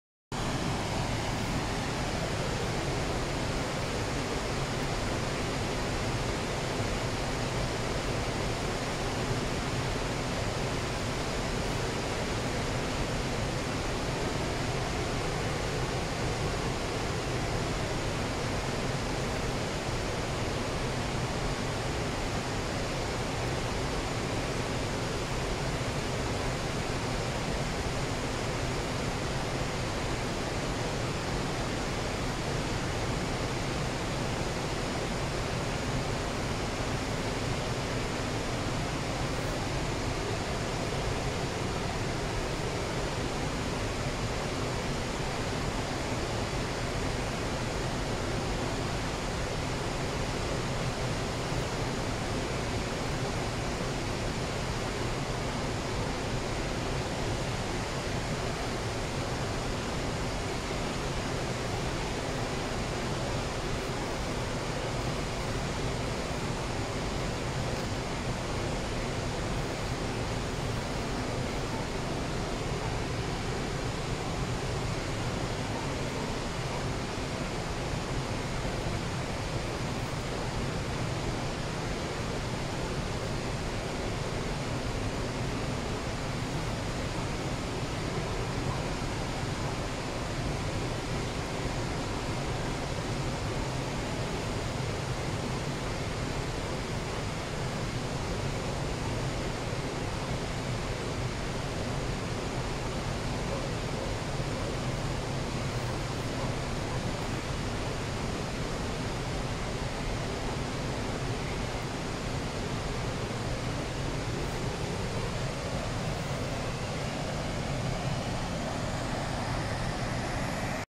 🏴󠁧󠁢󠁳󠁣󠁴󠁿😍🏴󠁧󠁢󠁳󠁣󠁴󠁿AMAZING SCOTLAND WATERFALL 🏴󠁧󠁢󠁳󠁣󠁴󠁿😍🏴󠁧󠁢󠁳󠁣󠁴󠁿
Short clip of the viewpoint at the falls of clyde 😍😍